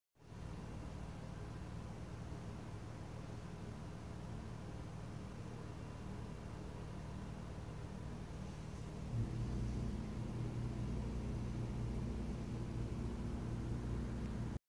Real Time Microscope Crystal Growth
A microscopic droplet of 3% Copper ii Acetate solution evaporating and crystals forming.

Microscope is set up as transmitted brightfield.